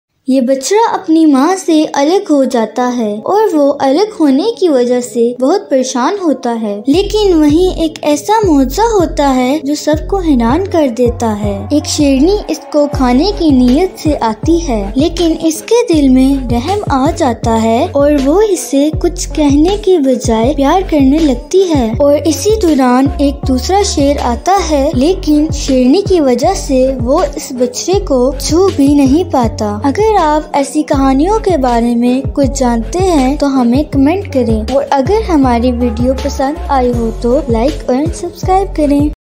tolerance of lioness
this is about calf who is lost.A lioness help him